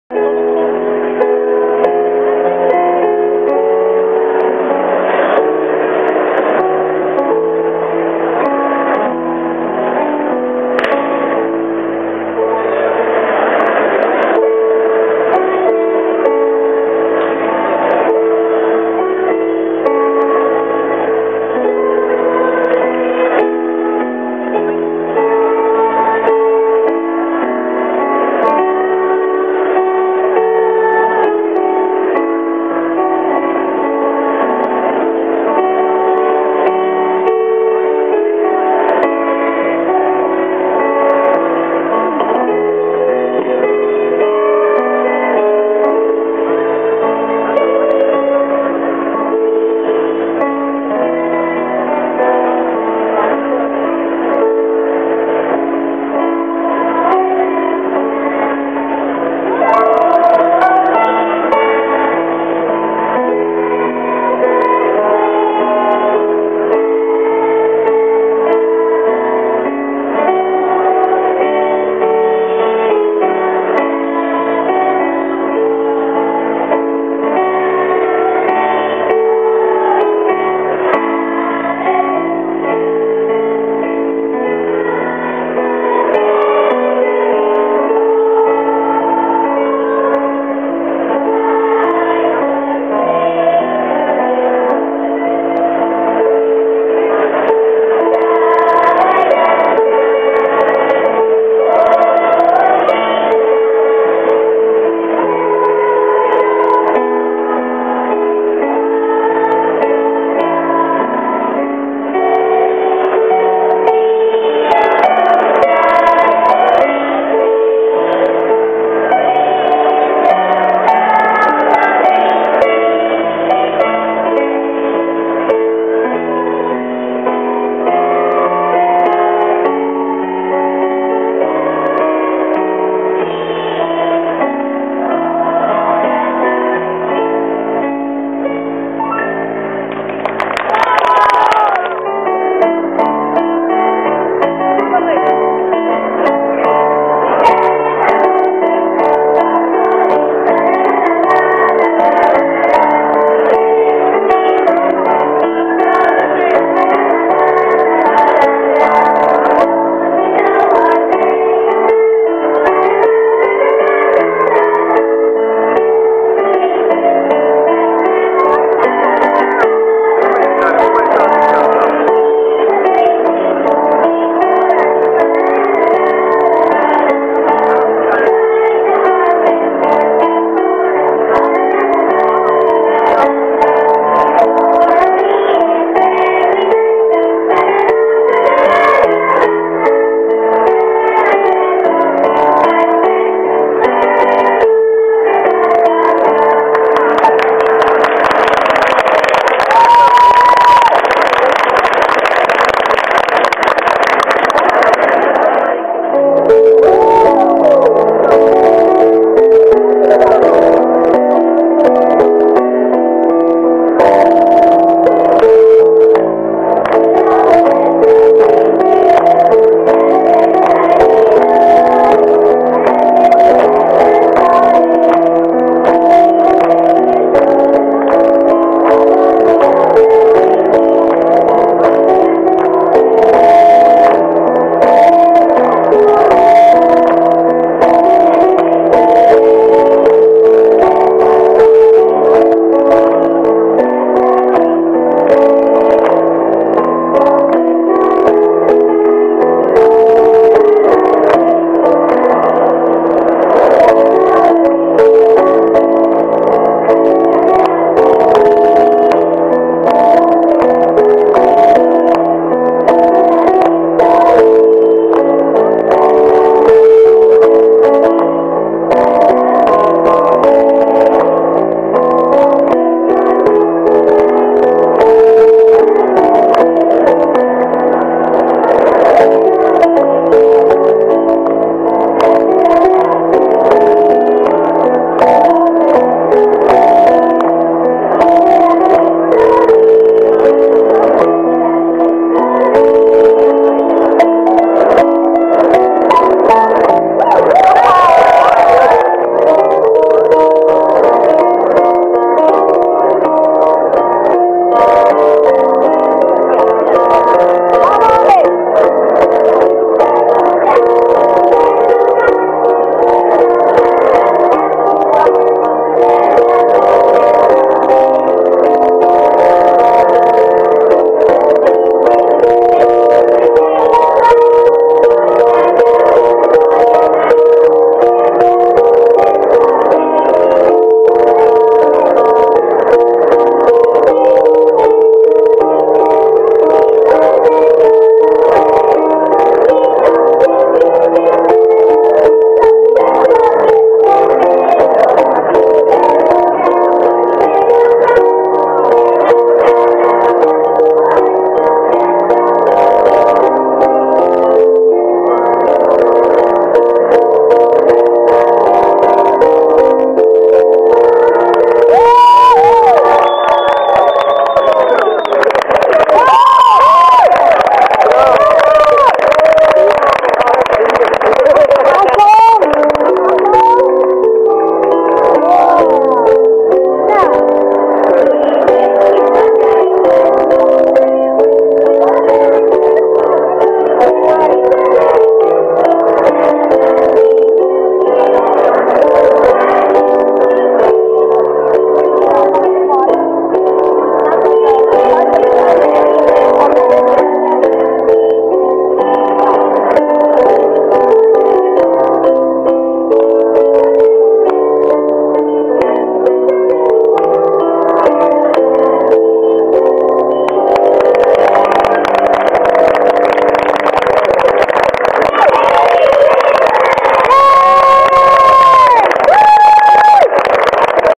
Prima Choir at Christmas bazaar
Prima International School Choir singing at the IWC Charity bazaar, Belgrade, 4 Dec 2011